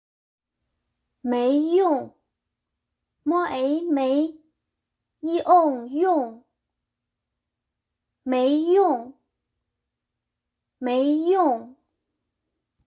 How to Pronounce USELESS in Mandarin Chinese | Learn Chinese HSK 3 Vocabulary
How to say useless in Mandarin Chinese?

How to pronounce useless in Mandarin Chinese?